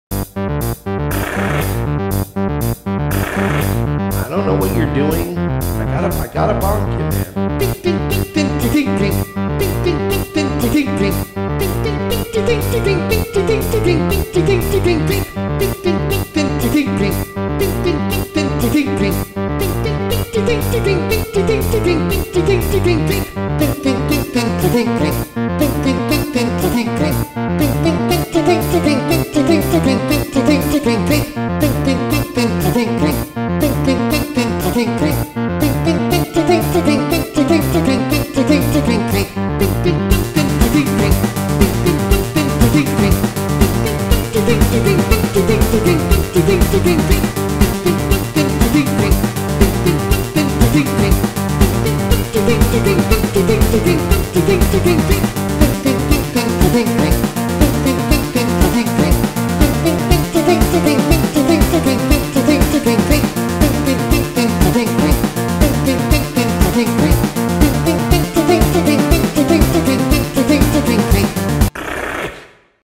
Mersh Popcorn
Today we get a musical rendition of our favorite single, plus sized, cat owner in Mersh Popcorn. please enjoy